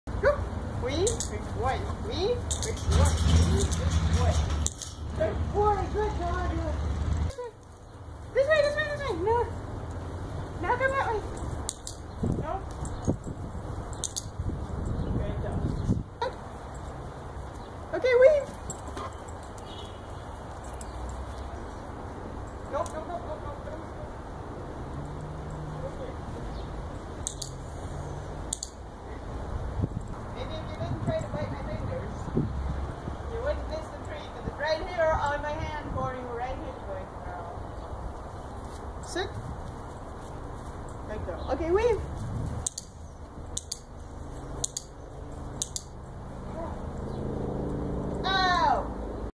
Dryfuss and Lucy weaving
Dryfuss is a little better at things that involve being patient and not just running forward at full speed

This is after about an hour of intermittent training (mostly with both dogs trying to figure it out together).